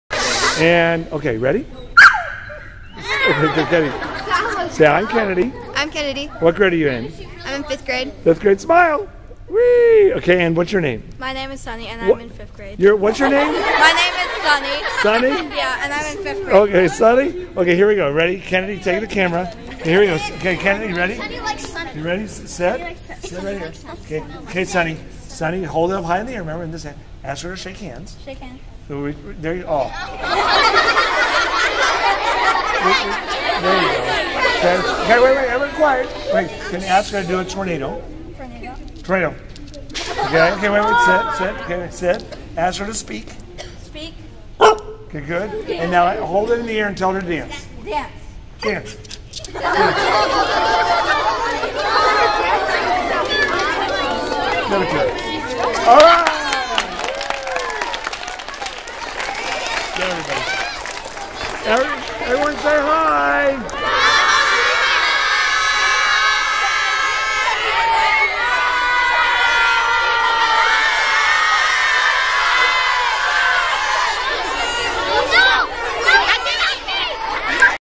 Sunny Visits Countryside Elementary
Sunny The Weather Dog visits her first school as a 1 year old!